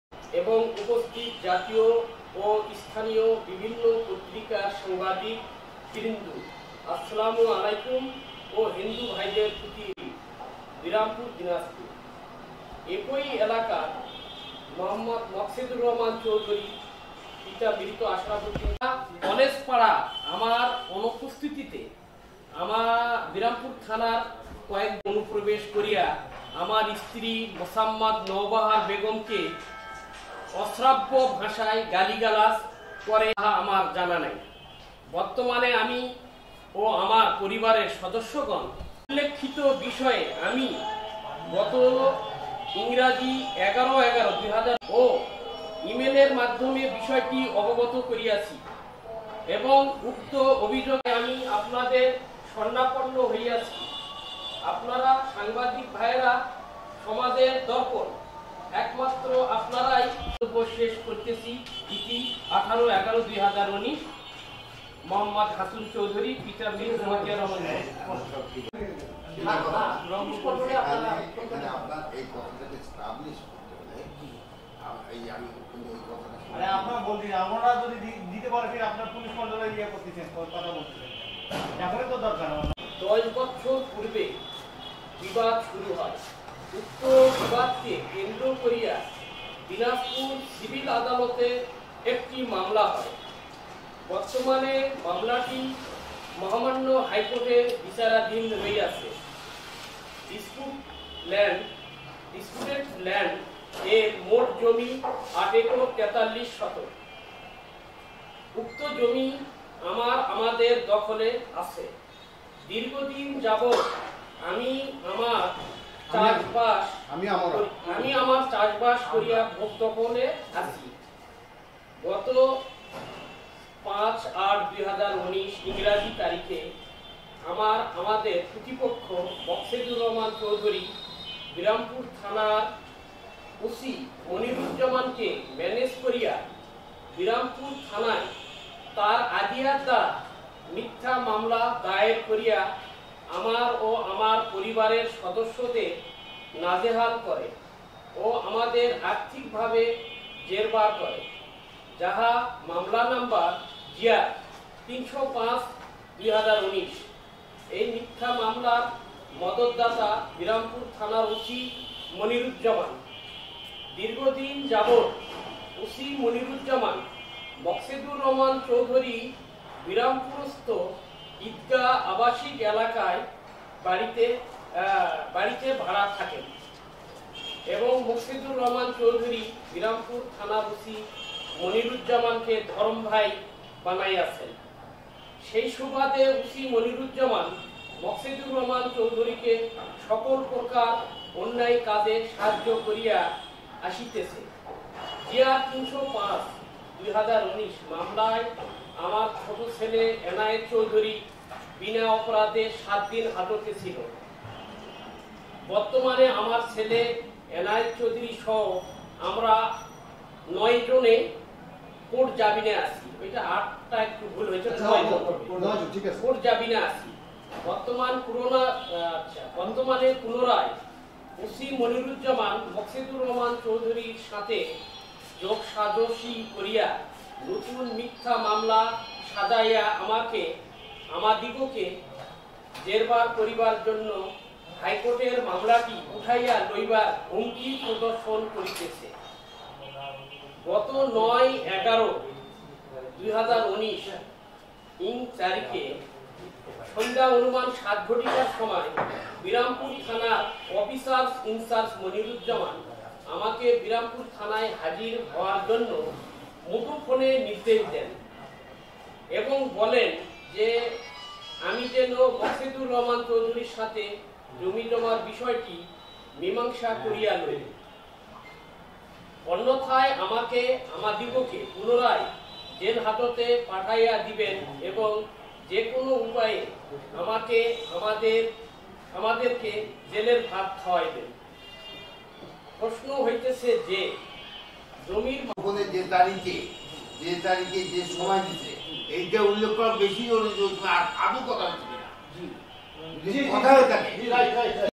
দিনাজপুর জেলার বিরামপুর থানার ওসির বিরুদ্ধে হয়রানী ও হুমকির অভিযোগ উঠেছে।
হাসুন চৌধুরী গ্রামের নাম একইর, বিরামপুর, দিমাজপুর। একই এলাকার ১। মকছেদুর রহমান চৌধুরী, পিভা- মৃত: আশরাফ চৌধুরী ও ২। আব্দুল্লাহ আল মাসুদ চৌধুরী, পিতা- মকছেদুর রহমান চৌধরী, সাং- একইর, ডাকষর- একইর, থানা- বিরামপুর, জেলা- দিনাজগুরের সহিত আমার/আমাদের জমা জমি লইয়া আনুমানিক ১০ বৎসর পূর্বে বিবাল শুরু হয়। উক্ত বিবাদকে কেন্দ্র করিয়া नিনাজপুর সিভিল আদালতে একটি মামলা হয়। বর্তমানে মামলাটি মহামান্য হাইকোটে বিচারাধীন রইয়াছে। ডিসপুটেড ল্যান্ড এর মােট জমি ৮.৪৩ একর। উক্ত জমি আমার/আমাদের দখলে আছে। দি্ঘী দিন যাবত আমি/আমরা চাষাবাদ করিয়া ভােগ দখলে আছি। গত ০৫/০৮/২০১৯ ইং তারিখে আমার/আমাদের প্রতিপক্ষ মকছেদুর রহমান চৌধুরী বিরামপুর থানায় ওসি মানিরুজ্জামান কে ম্যানেজ করিয়া বিরামপুর থানায় তাঁজাধিয়ার দা মিথ্যা মামলা দায়ের করিয়া আমার ও অমির পরিবারের সদস্যদের নাজেহাল করে ও অমাদের আথিক ভাবে জের বার করে। যাহার মামলা নং জি/আর ৩০৫/১৯। এই মিথ্যা মামলার মদত দাতা বিরামপুর থানার ওসি মনিরুজ্জামান। দীর্ঘ দিন যাবত ওসি মনিরুজ্জামান মকছেদুর রহমান চৌধুরীর বিরামপুরস্থ ঈদগা আবাশিক এলাকার বাড়ীতে ভাড়া থাকেন এবং মকছেদুর রহমান চৌধুরী বিরামপুর থানার ওসি মানিরুজ্জামান কে ধরম ভাই বানাইয়াছেন। সেই সুবাদে ওসি মনিরুজ্জামান মকছেদুর রহমান চৌধুরীকে সকল গ্রকার অন্যায় কাজে সাহায্য করিয়া আসিতেছে। ডি,আর ৩০৫/১৯ মামলায় আমার ছেটি ছেলে এমায়ে চৌধুরী বিনা অপরধে ৭ দিন হজঅতে হিল। বর্তমানে আমার ছেলে এনায়েক চৌধুরী সহ আমরা মােট ০৮ জন কোট জামিনে আছি। বর্তমানে পূনরায় ওসি মানরুজ্জামান মকছেদুর রহমান চৌধুরীর সাথে যােগসাজসি করিয়া নতুন মিথ্যা মামলা সাজাইয়া আমাকে/আমাদিগকে জেরবার করিবার জন্য হাইকোর্টের মামলাটি উঠাইয়া লইবার হুমকি প্রদর্শ করিজেছে। গত ০৯/১১/২০১৯ ইং তারিখে সন্ধা অনুমান ৭.০০ টার সময় বিরামপুর থানার অকিলার ইনচার্জ মূনিরুজ্জামান আমাকে বিরামগুর থানায় হাজির ইওয়ার জন্য মুঠোফোনে নির্দেশ দেন এবং বলেন যে, আমি যেন মকছেদুর রহুমান চৌধুরীর সাথে জমি জমার বিষয়টি মিমাংশ করিয়া লই। অন্যথায় আমাকে/আমাদিগকে পুনরায় জেল হাজতে গাঠাইয়া দিবেন এবং যে কোন উপায়ে আমাকে আমাদের কে জেলের ভাত খাওইবেন। প্রশ্ন হইতেছে যে, জমির মামলাটি মহামান্য হাইকোট বর্তমানে বিচারাধীন রহিয়াছে। লে ক্ষেত্রে মনির্জ্জামান ওসি কি করিয়া মামলাটি উঠাইয়া লইতে বলেন ? তাহা আমার জনা নহি। আরও উল্লেখ করা আবশ্যকে যে, ৯/১১/২০১৯ তারিখে বিরামপুর থানার ওসির নির্দেশ ক্রমে রাত্রি অনুমান ৮টার সময় বিরামপুর থানার কলেজ পাড়ায় আমার অনুপস্থিতে বিরামপুর থানার কয়েকজন পুলিশ থানার পিকাপ ভ্যানে গিয়া আমায় বাড়ীতে অনুপ্রবেশ করিয়া আমার ট্ত্রী মােছা: নওবাহার বেগম কে অশ্রাব্য ভাথার গাঁলি গালাজ করে এবং ঘরের আসবাব পত্র তছনছ করে। বিরামপুর থানা পুলিশ কোন অদৃশ্য শক্তির বলে উক্ত কাজটি করিয়াছে, তাহা আমার জানা নাই। বর্তমানে আমি ও আমার পরিবারের সদস্যগণ ওসি মনিরম্জ্জামানের ছমকির কারনে আমি/আমরা পাড়া হইতে বাহির হইতে পারিতেছি না। অর্থাৎ পরিবারের সদস্যসের দের আমার যােগাযােগ বিচ্ছিন্ন রহিয়াছে। তাবে মুঠো ফোনে রহিয়াছে। ইহা সত্য যে, রাস্তায় আমাকে একাকী দেখা পাইলে গসি মনিরুজ্জামান পূর্বের ন্যায় মিথ্যা মামলায় আমাকে আাটক করিয়া সন্ত্রাসি মামলা দিয়া অথবা মাদক দ্রব্য আমার নিকট পাওয়া নিরাছে মর্মে উল্লেখ করিয়া মিথ্যা মামলা রজু করিরা আমাকে কোট হাজতে প্রেরন করিবে। বিশেষ ভাবে উল্লেখ্য যে, উপরে উল্লেখিত বিষয়ে আমি গত ১১/১১/২০১৯ ইং তারিখে দিনাজপুর গুলিশ সুগার সাহেবের নিকট ডাকযােগে ও ই-মেইলের মাধ্যমে বিষয়টি আবগত করিয়াছি এবং উক্ত অভিযােগের অনুলিপি ১। অতিরিক্ত ডি,আই,ডি (ডিএসপিএস-১), পুলিশ হেড কোয়াটার্স, ঢাকাকে লিখিত আকারে ই-মেইল ও ডাকযােগে অবগত করিয়াছি। সেই সাথে বিষয়টি বিভাগীয় পুলিশ কর্মকর্তা (ডিআইজি), রংপুরকে ডাকযােগে অবগত করিয়াছি। আমি পুলিশ বিভাগের উর্দ্ধতন কর্তকর্তাগনের সাহায্য ও হস্তক্ষেপ এবং নেক নজর কামনা করিতেছি। তবেই পুলিশ বাহিনীর ঐ স্লোগানটি সঠিক বলিয়া বিবেচিত হইবে যে, "পুলিসই জনতা জনতাই পুলিশ।" দীর্ঘ সময় ধরে আমার লিখিত বক্তব্য আপনারা সাংবাদিক ভাই'রা শুনলেন আমি বিরামপুর থানার পুলিশ কর্মকর্তা ওসি মনিরুজ্জামানের রােযানল হইতে বাঁচতে চাই। তাই আমি আপনাদের স্বরাণাপন্ন হইয়াছি। আপনারা সংবাদিক ভাইরা সমাজের দৰ্গন। একমাত্র আপনারাই পারেন। আপনাদের লিখনি শক্তি দিয়া সঠিক বিষয়টি তুলে ধরতে। আমি বিরামপুর থানার ওসির বিচার চাই। বিরামপুর থানার ওসি মনিরুজ্জামান কেন বার বার আমার/আমাদের প্রতি জুলুম ও অন্যায় অত্যচার করিতেছে, সে ব্যবাপায়ে আমি বিরামপুর উপজেলার সুধি সমাজের ও গুলিশের উর্দ্ধতন কর্মকর্তাদের হস্তক্ষেপ এবং সুদৃষ্টি কামনা করিতেছি। পরিশেষে আপনাদের সকলের সুন্বাস্থ ও দীর্ঘায়ু কামনা করে আমার লিখিত বক্তব্য শেষ করছি।